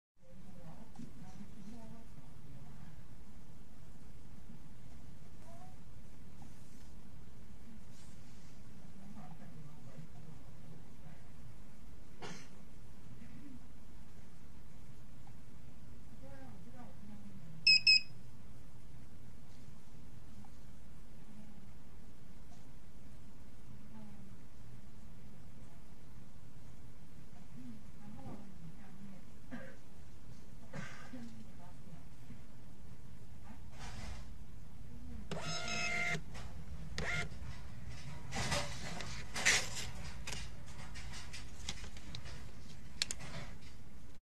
【Wireless Receipt Printing Demo】 : Birch P8100P tablet and Q5-41 Bluetooth receipt printer
This video: Using P8100P tablet and do wireless printing in  Q5-41 Bluetooth receipt printer

1. P8100P: 10.1 inch industry Rigid tablet.
2, Q5-41: 3 inch receipt printer, USB+LAN+ RS232+Bluetooth
3. App :【4BarcodeEdit App】